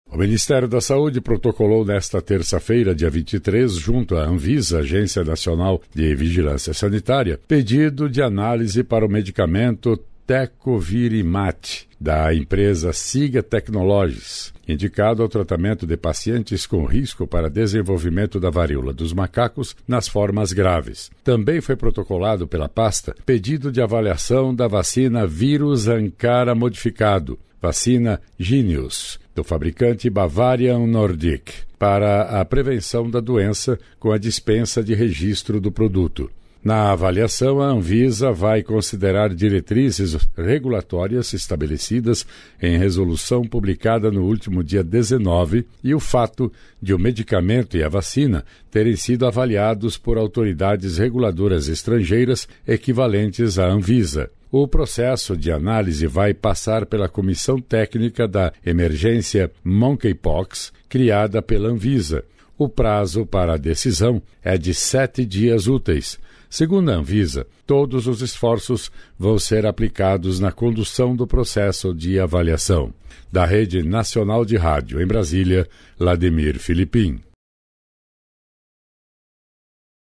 24/8/2022 - ANVISA AVALIA USO DE ANTIVIRAL E VACINA CONTRA VARÍOLA DOS MACACOS
Pedido foi feito pelo Mistério da Saúde e avaliação vai passar pela Comissão Técnica da Emergência Monkeypox. Prazo para a decisão é de 7 dias úteis.

O Ministério da Saúde protocolou nessa terça-feira (23), junto à Anvisa, Agência Nacional de Vigilância Sanitária, pedido de análise para o medicamento tecovirimat, da empresa SIGA Technologies, indicado ao tratamento de pacientes com risco para desenvolvimento da Varíola dos Macacos nas formas graves.

Também foi protocolado pela pasta pedido de avaliação da Vacina Vírus Ankara Modificado, vacina Jynneos, do fabricante Bavarian Nordic, para a prevenção da doença, com a dispensa de registro do produto.

Na avaliação, a Anvisa vai considerar diretrizes regulatórias estabelecidas em resolução publicada no último dia 19 e o fato de o medicamento e a vacina terem sido avaliados por autoridades reguladoras estrangeiras equivalentes à Anvisa.

O processo de análise vai passar pela Comissão Técnica da Emergência Monkeypox, criada pela Anvisa.

O prazo para a decisão é de 7 dias úteis. Segundo a Anvisa, todos os esforços vão ser aplicados na condução do processo de avaliação.